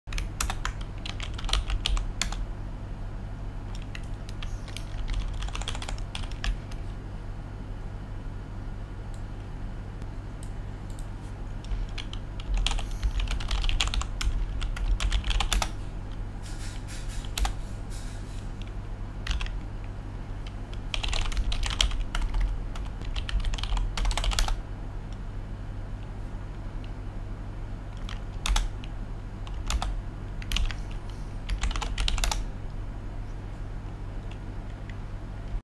How to Fix Broken Python Program
How to fix broken python program that doesn't start - pip and pacman - Arch Linux Tutorial